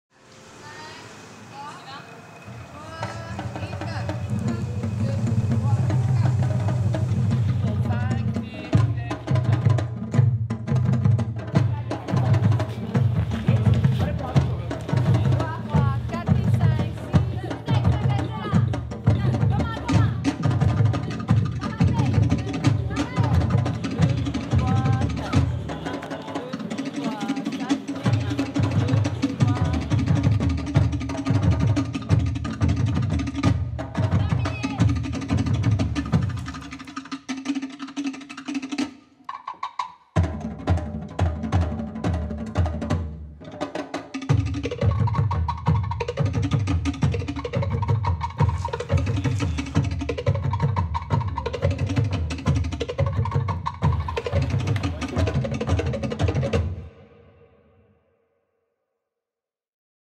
Tamari'i Tipaerui - Extract 1 - Documentary tahitian dance
Exctract 1 of tahitian dance documentary about preparation for the Heiva 2014.